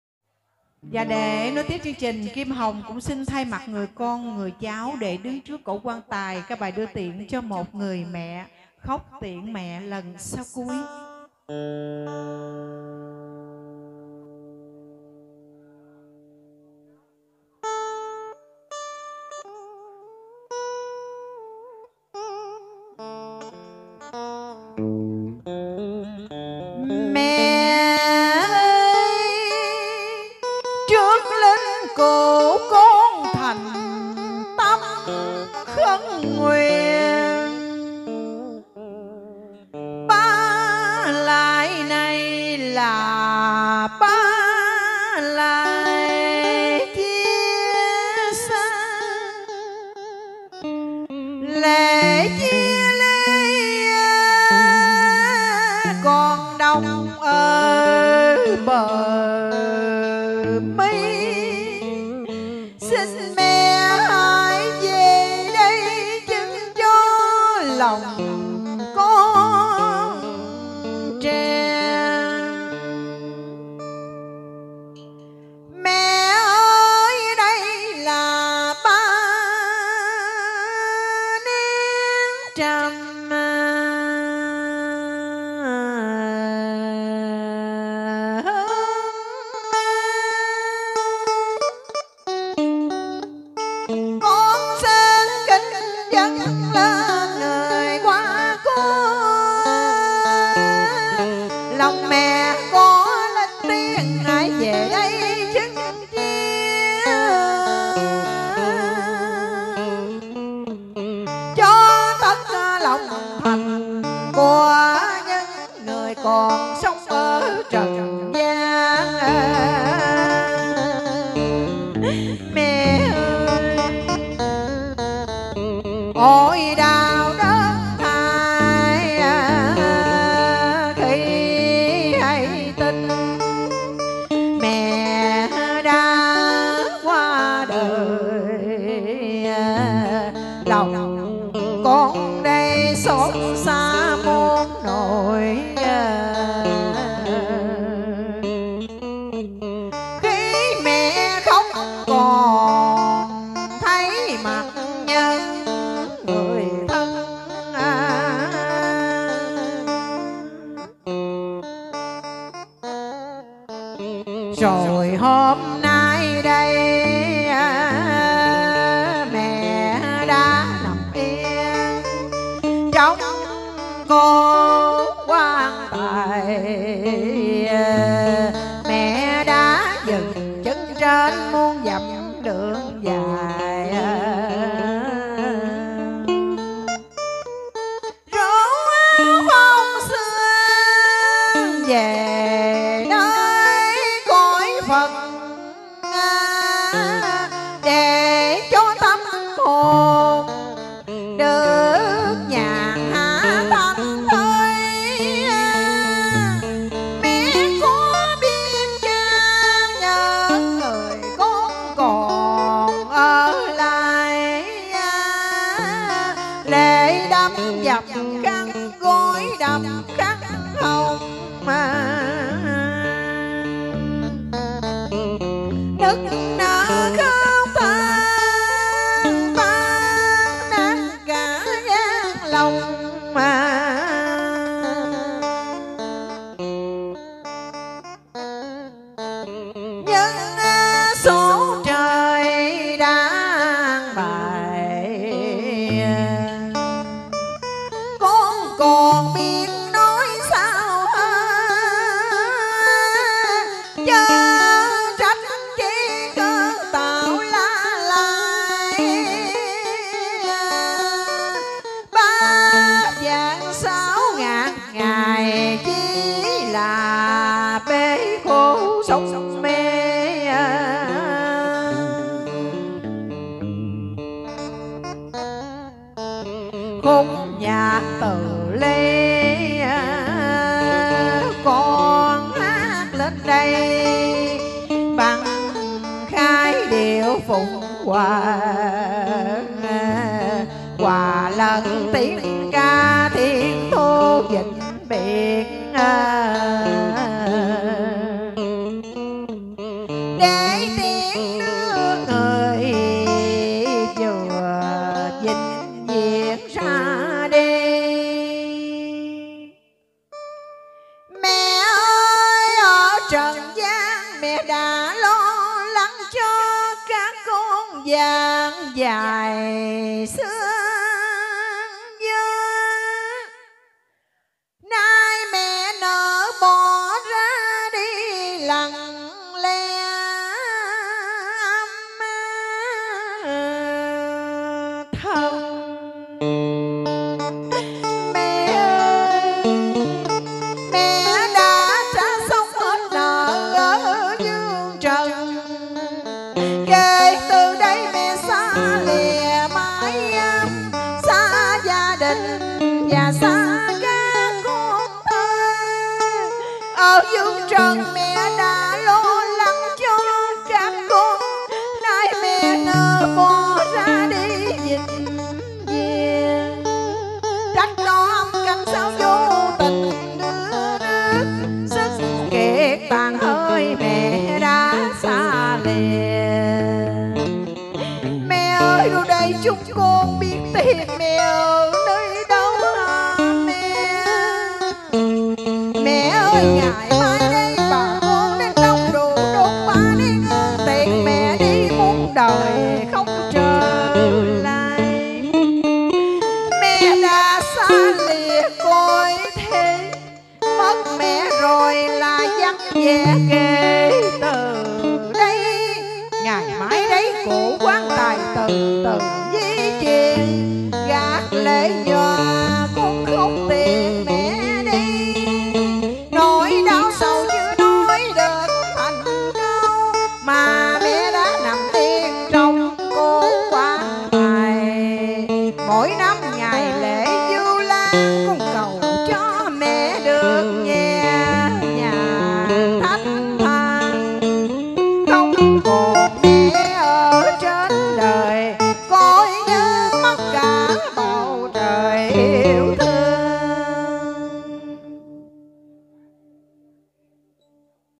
Ca Cúng Có Khi Phải Quay Mặt Ra Ngoài Như Thế Này Giống Kim Hồng Cho Khán Giả Họ Xem..
Các bạn đi hát ca cúng đừng bao giờ quay mặt vào trong hoài mà hát.Lâu lâu phải quay mặt ra ngoài để cho khán giả họ xem mình hát như vậy khách họ mới thích thú và nghe hát.